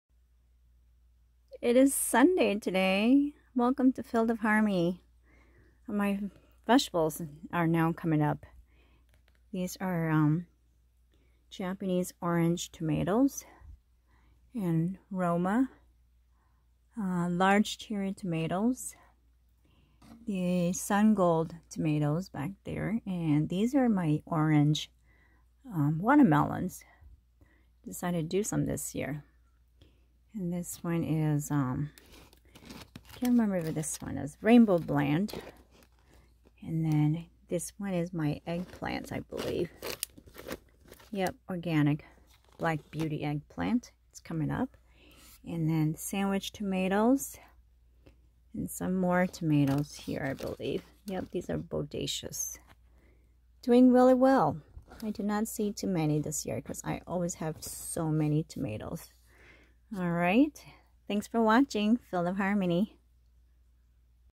Vegetable seedlings!
Tomatoes, peppers, watermelons,.and eggplants!  I am running out of shelf space once again 😁